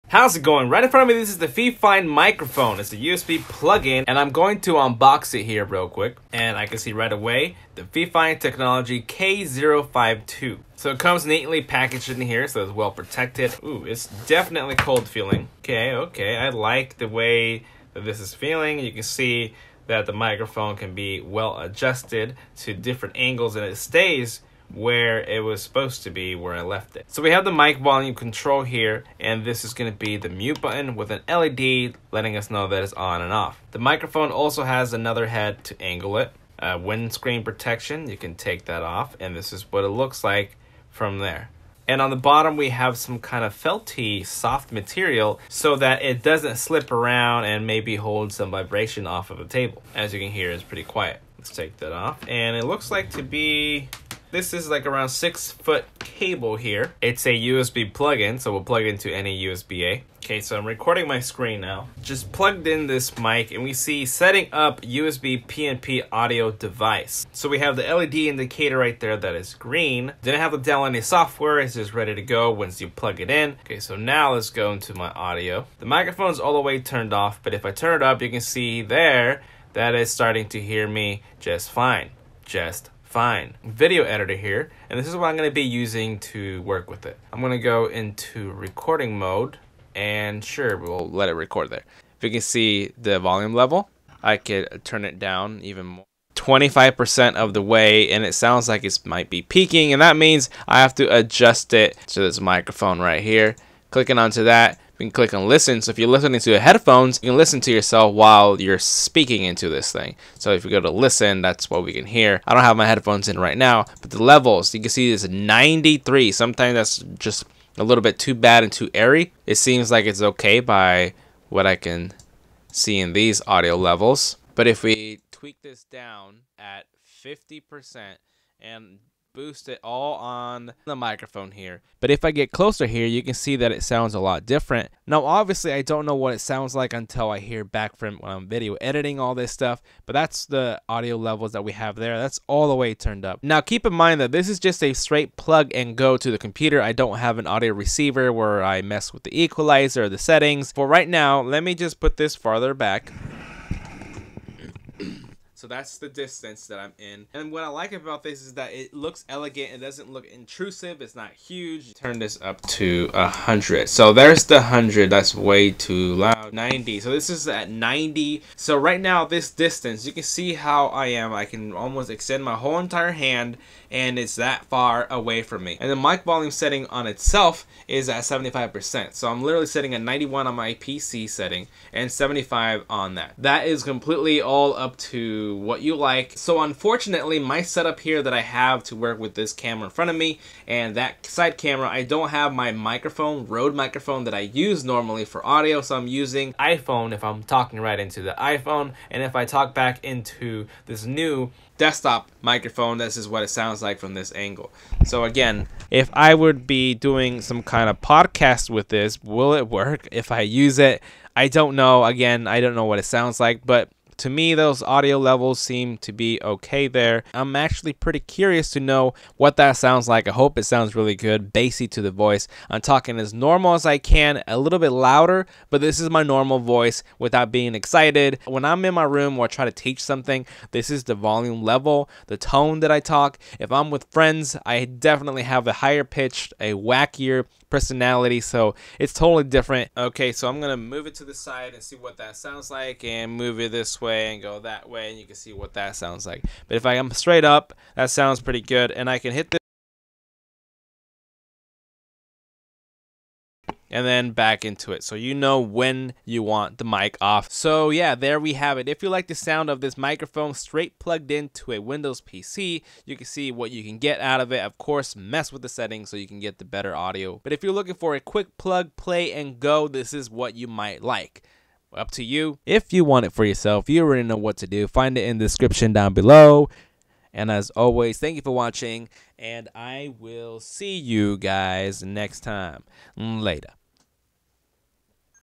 Desktop Gooseneck USB Microphone - FiFine
To my surprise, I found this K052 Mic to sound really good. I could definitely use this for gaming and voice-overs. Take a listen!

3. All in 1 Portable Charger

9. Mid Tower PC Case